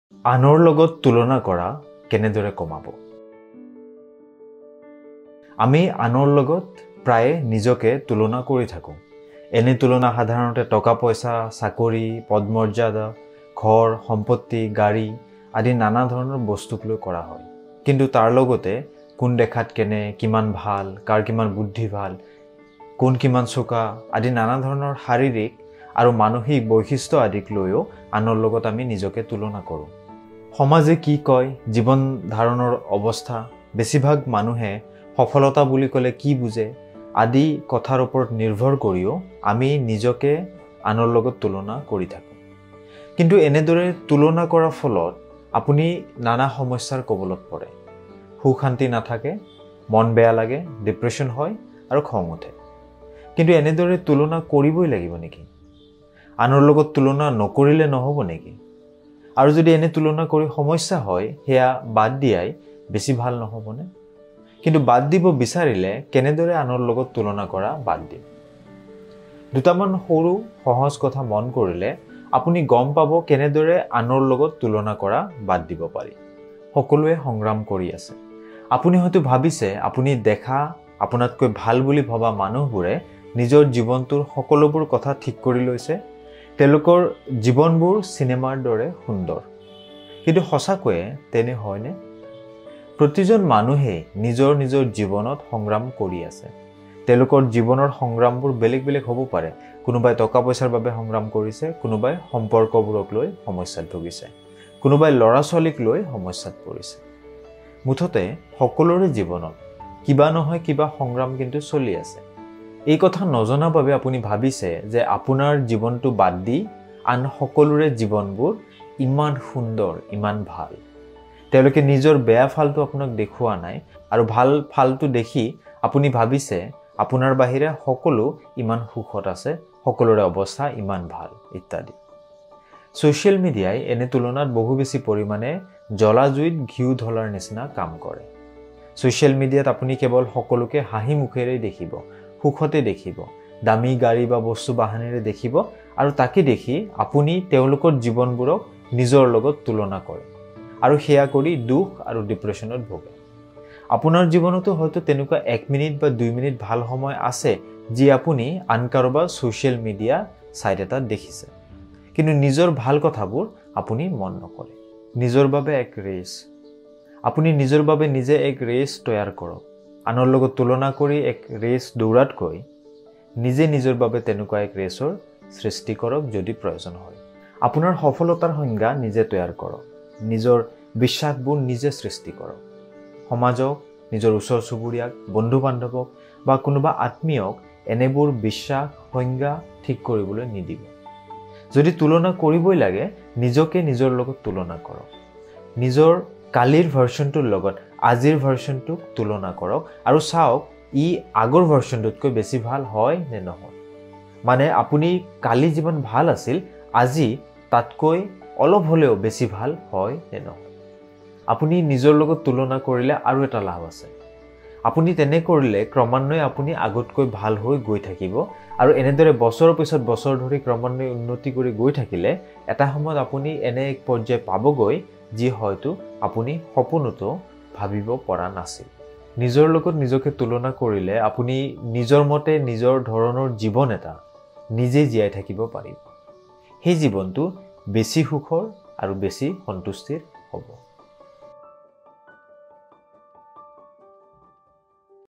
আনৰ লগত তুলনা কৰা কেনেদৰে কমাব? | How to stop comparing with others | Bikash Kalita
আমি আনৰ লগত প্ৰায়ে নিজকে তুলনা কৰি থাকো৷ এনে তুলনা সাধাৰণতে টকা-পইচা, চাকৰি, পদমৰ্যাদা, ঘৰ, সম্পত্তি, গাড়ী আদি নানা ধৰণৰ বস্তুক লৈ কৰা হয়৷ কিন্তু তাৰ লগতে কোন দেখাত কেনে, কিমান ভাল, কাৰ কিমান বুদ্ধি ভাল৷ কোন কিমান চোকা, আদি নানা ধৰণৰ শাৰীৰিক আৰু মানসিক বৈশিষ্ট আদিক লৈও আনৰ লগত আমি নিজকে তুলনা কৰোঁ৷

কাউঞ্চেলিঙৰ জৰিয়তে সমস্যাৰ লগত যুঁজিবলৈ আৰু জীৱনটো আগতকৈ ভাল কৰিবলৈ অলপ সহায় আৰু চাপৰ্ট লাভ কৰক

Online Courses
অসমীয়াত অনলাইন কৰ্ছ